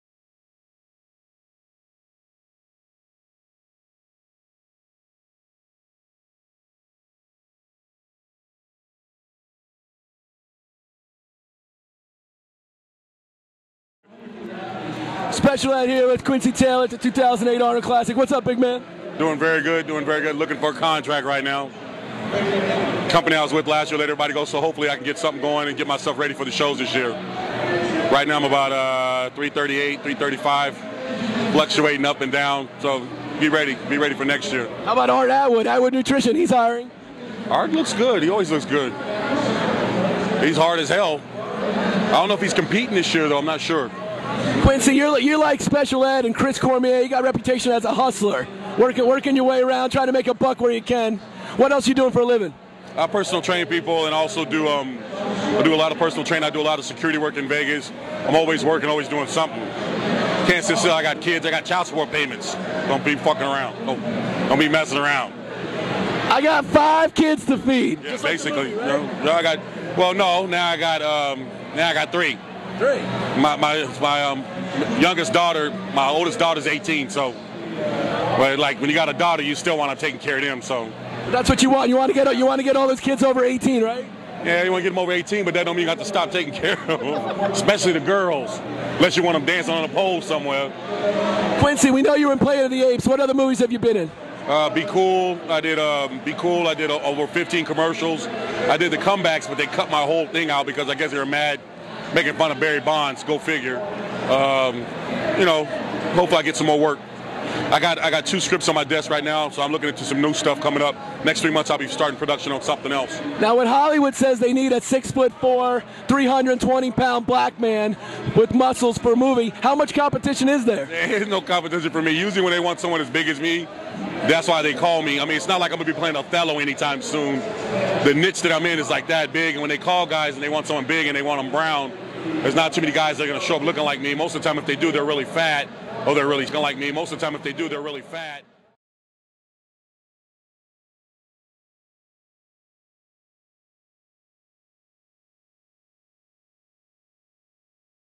IFBB Pro Bodybuilder QUINCY TAYLOR (Part I)
Special Ed interviews Massive 340 lb. Professional Bodybuilder Quincy Taylor about how a man his size earns a living. Answer: Any way he wants!